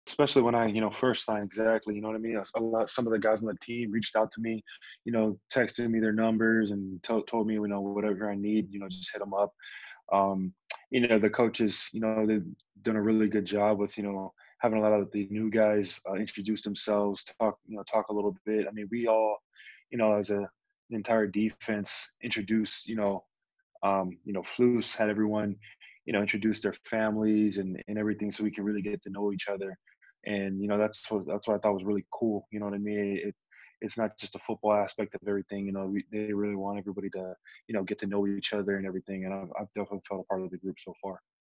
DeForest Buckner at home with the Colts
First-year Colts defensive tackle DeForest Buckner says he's been welcomed into his new team, albeit virtually.